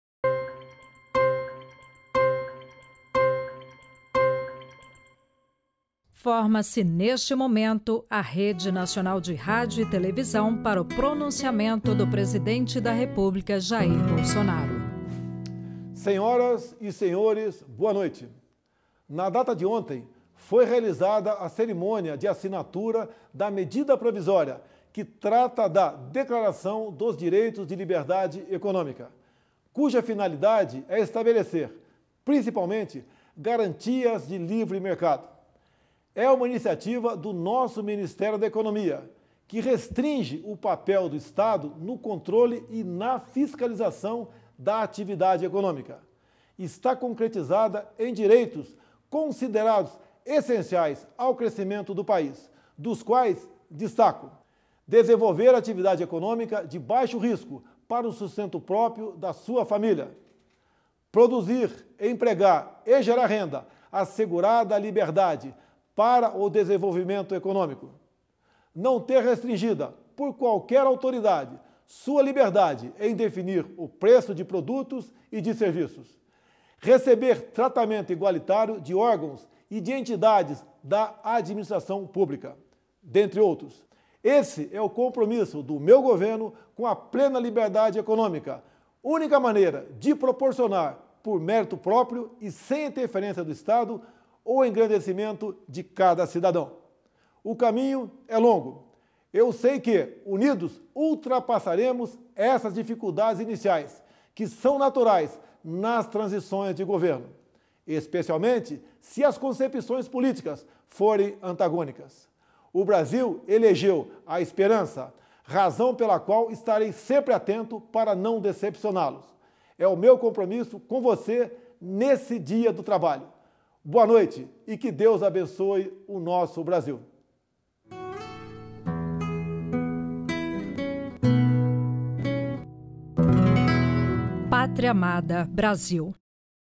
Pronunciamento do Excelentíssimo Senhor Presidente da República, Jair Bolsonaro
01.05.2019 - O Presidente da República destacou a Medida Provisória, assinada nesta semana, que trata da liberdade econômica para garantia de livre mercado.